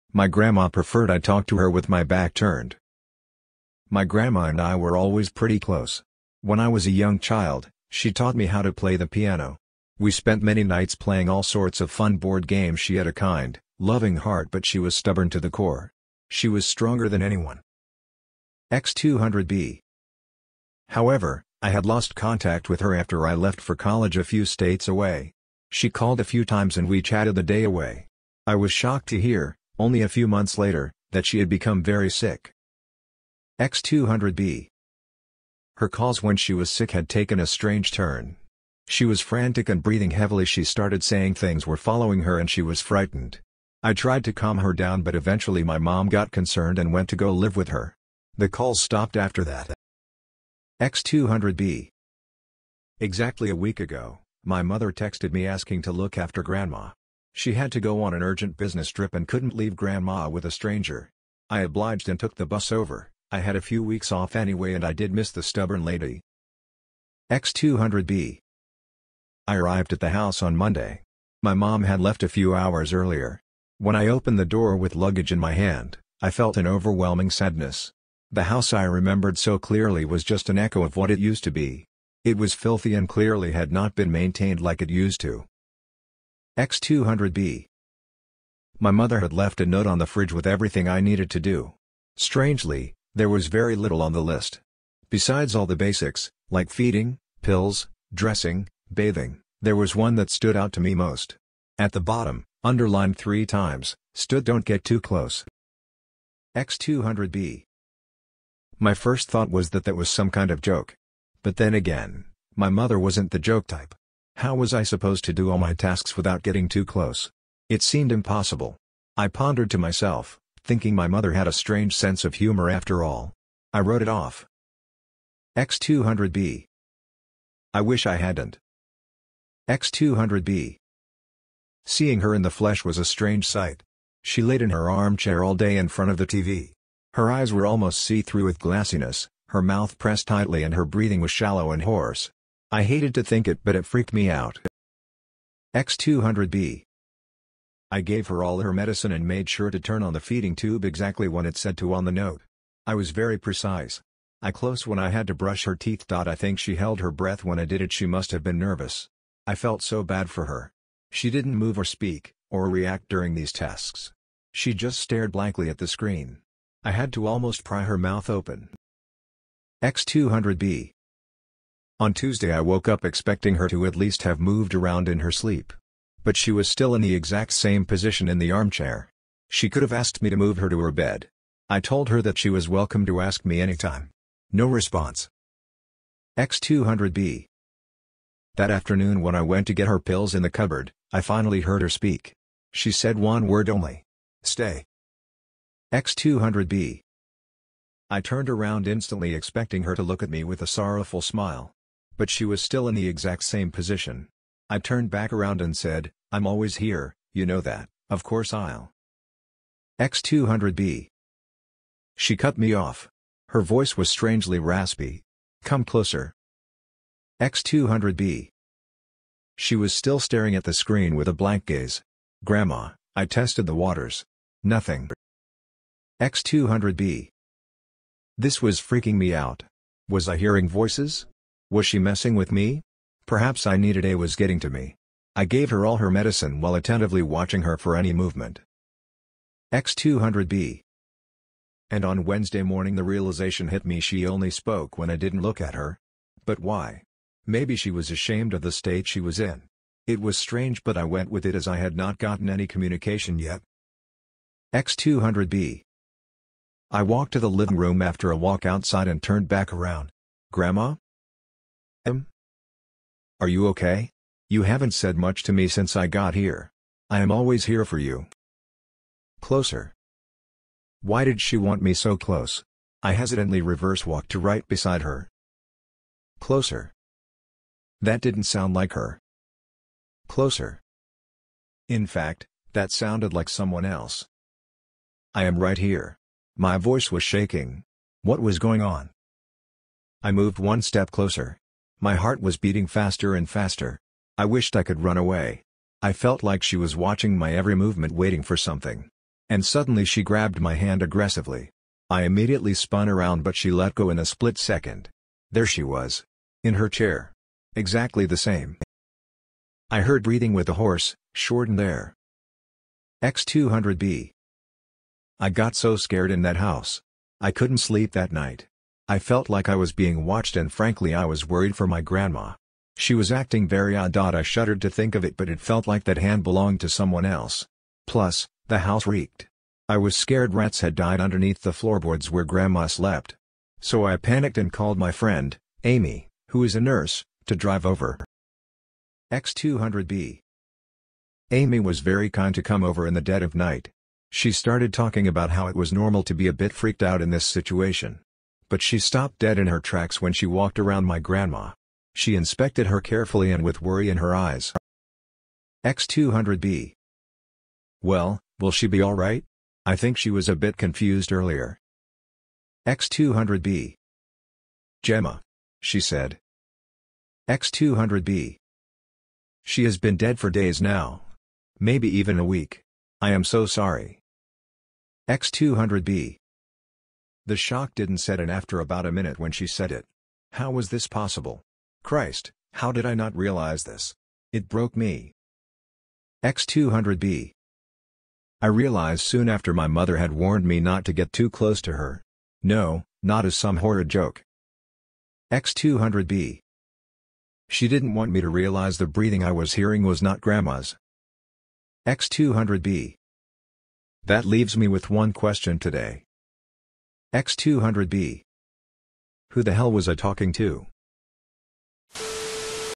My grandma preferred I talk to her with my back turned.
My grandma and I were always pretty close. When I was a young child, she taught me how to play the piano.  We spent many nights playing all sorts of fun boardgames-she had a kind, loving heart but she was stubborn to the core. She was stronger than anyone.

However, I had lost contact with her after I left for college a few states away. She called a few times and we chatted the day away. I was shocked to hear, only a few months later, that she had become very sick.

Her calls when she was sick had taken a strange turn. She was frantic and breathing heavily; she started saying things were following her and she was frightened. I tried to calm her down but eventually my mom got concerned and went to go live with her. The calls stopped after that.

Exactly a week ago, my mother texted me asking to look after grandma. She had to go on an urgent business trip and couldn't leave grandma with a stranger. I obliged and took the bus over, I had a few weeks off anyway and I did miss the stubborn lady.

I arrived at the house on Monday. My mom had left a few hours earlier. When I opened the door with luggage in my hand, I felt an overwhelming sadness. The house I remembered so clearly was just an echo of what it used to be. It was filthy and clearly had not been maintained like it used to.

My mother had left a note on the fridge with everything I needed to do. Strangely, there was very little on the list. Besides all the basics, like feeding, pills, dressing, bathing, there was one that stood out to me most. At the bottom, underlined 3 times, stood :  "Don't get too close."

My first thought was that that was some kind of joke. But then again, my mother wasn't the joke type. How was I supposed to do all my tasks without getting too close? It seemed impossible. I pondered to myself, thinking my mother had a strange sense of humour after all. I wrote it off.

I wish I hadn't.

Seeing her in the flesh was a strange sight. She layed in her armchair all day in front of the TV. Her eyes were almost see-through with glassiness, her mouth pressed tightly and her breathing was shallow and hoarse. I hated to think it but it freaked me out.

I gave her all her medicine and made sure to turn on the feeding tube exactly when it said to on the note. I was very precise. I got...very close when I had to brush her teeth.I think she held her breath when I did it- she must have been nervous. I felt so bad for her.  She didn't move or speak, or react during these tasks. She just stared blankly at the screen. I had to almost pry her mouth open.

On Tuesday I woke up expecting her to at least have moved around in her sleep. But she was still in the exact same position in the armchair. She could've asked me to move her to her bed. I told her that she was welcome to ask me anytime. No response.

That afternoon when I went to get her pills in the cupboard, I finally heard her speak. She said one word only. "Stay."

I turned around instantly expecting her to look at me with a sorrowful smile. But she was still in the exact same position. I turned back around and said, "I'm always here, you know that, of course I'll-"

She cut me off. Her voice was strangely raspy. "Come closer."

She was still staring at the screen with a blank gaze. "Grandma," I tested the waters. Nothing.

This was freaking me out. Was I hearing voices? Was she messing with me? Perhaps I needed a break...this was getting to me. I gave her all her medicine while attentively watching her for any movement.

And on Wednesday morning the realisation hit me-she only spoke when I didn't look at her. But why? Maybe she was ashamed of the state she was in. It was strange but I went with it as I had not gotten any communication yet.

I walked to the livkng room after a walk outside and turned back around. "Grandma?"

"Mm?"

"Are you OK? You haven't said much to me since I got here. I am always here for you."

"Closer."

Why did she want me so close? I hesitantly reverse walked to right beside her.

"Closer."

That didn't sound like her.

"Closer."

In fact, that sounded like someone else.

"...Grandma. I am right here." My voice was shaking. What was going on?

I moved one step closer. My heart was beating faster and faster. I wished I could run away. I felt like she was watching my every movement waiting for something. And suddenly she grabbed my hand aggressively. I immediately spun around but she let go in a split second. There she was.. in her chair. Exactly the same.

I heard breathing with a hoarse, shortened air.

I got so scared in that house. I couldn't sleep that night. I felt like I was being watched and frankly I was worried for my grandma. She was acting very odd.I shuddered to think of it but it felt like that hand belonged to someone el